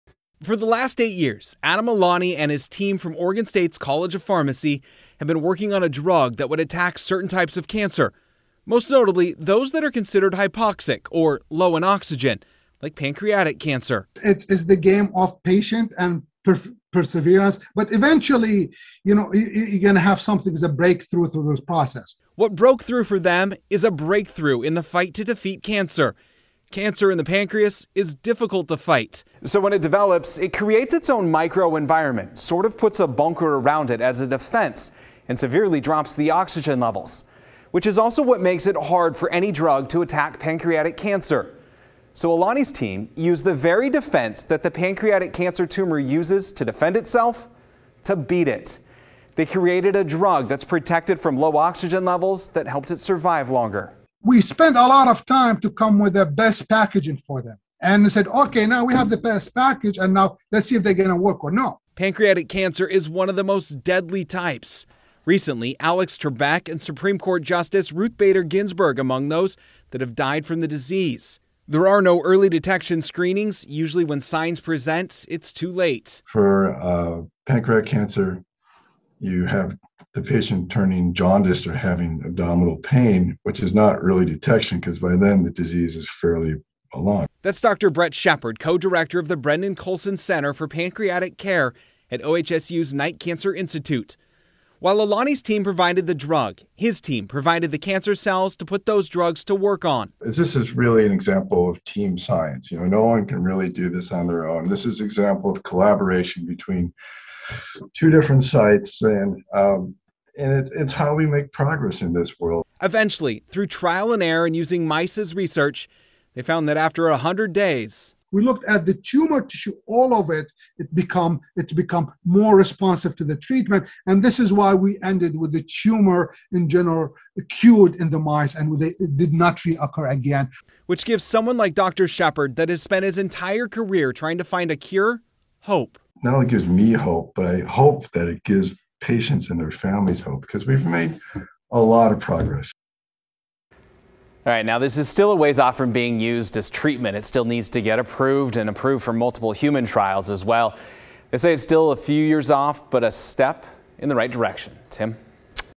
OSU team develops new potential treatment for pancreatic cancer
A team of scientists at both Oregon State University's College of Pharmacy and at the Brenden-Colson Center for Pancreatic Care at OHSU's Knight Cancer Institute have developed a new therapy that, if approved, could significantly increase a patient's odds of surviving pancreatic cancer.